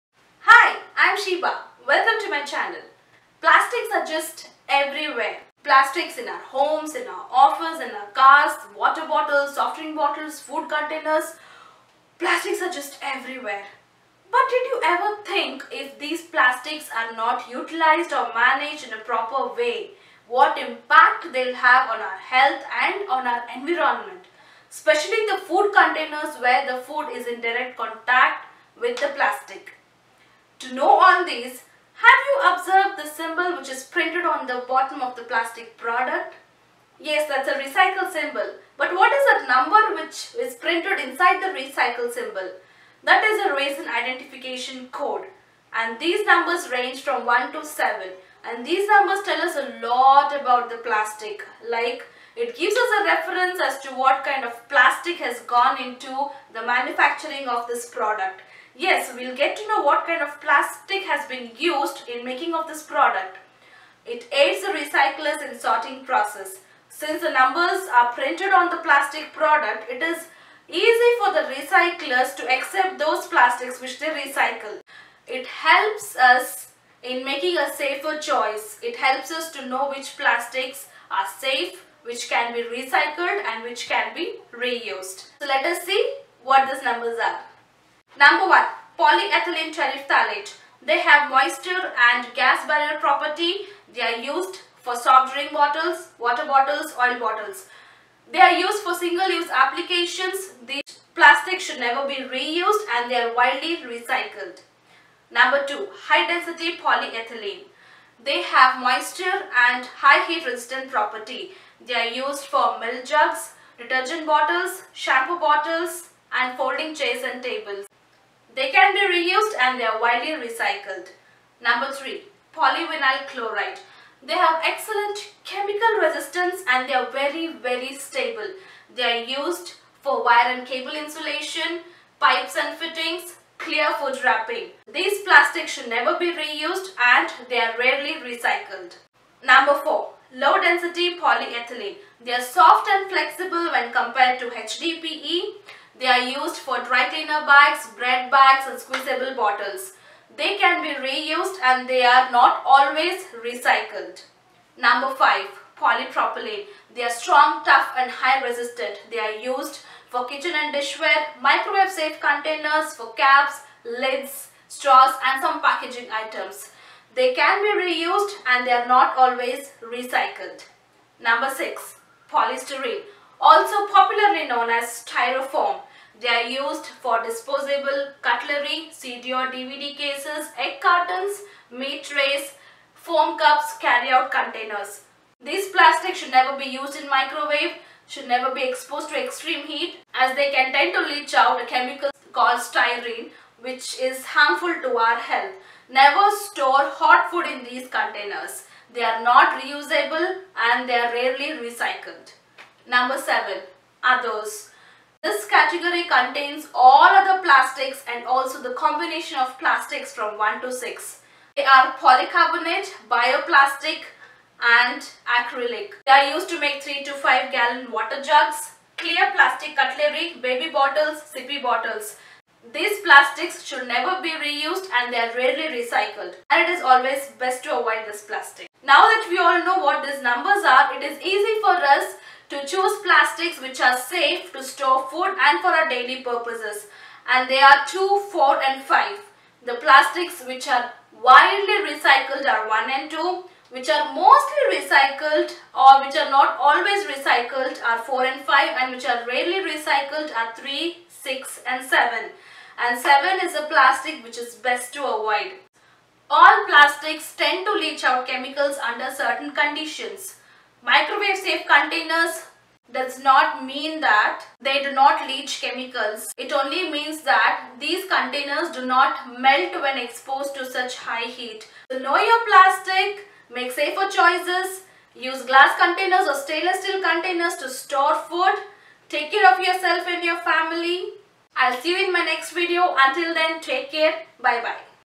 Know Your Plastic!!! Have you ever noticed this? Resin Recycle Number!
We know that Plastics are not decomposed and hence they are just piling up on our Planet. Recycling is one way to reduce the harmful effect caused by Plastics, but we cannot even completely avoid as it is highly impractical.
But we can contribute to save Earth to avoid using Plastics wherever it is possible. We can use Reusable Glass Containers or Stainless Steel Containers to store food and beverages as Plastics leach when they are exposed to certain conditions.
Resin Identification Code helps us to identify those Plastics which are safe, which can be reused and recycled.